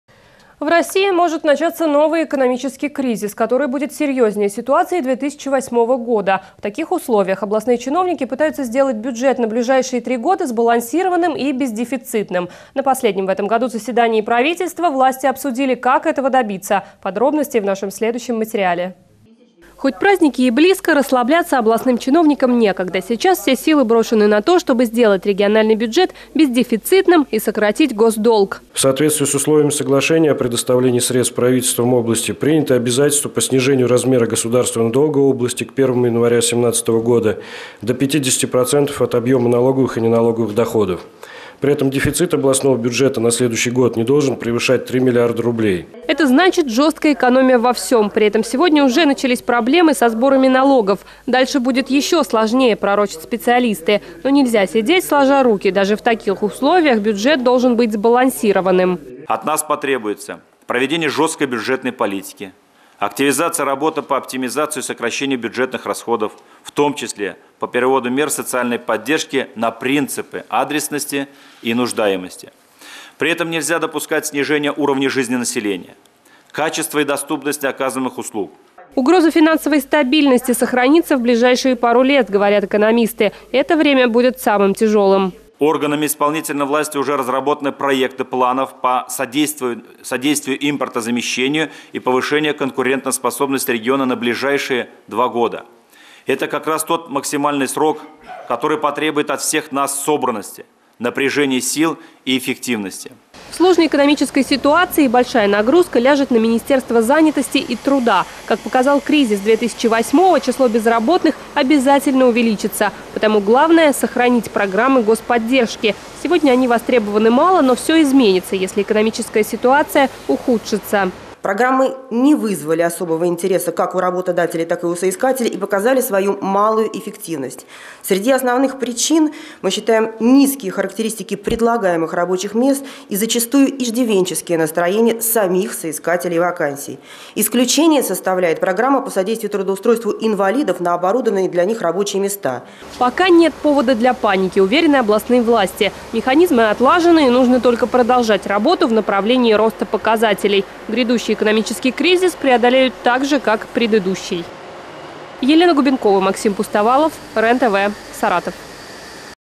В России может начаться новый экономический кризис
В таких условиях областные чиновники пытаются сделать бюджет на ближайшие 3 года сбалансированным и бездефицитным. На последнем в этом году заседании Правительства власти обсудили, как этого добиться. Подробности в нашем следующем материале от 25 декабря.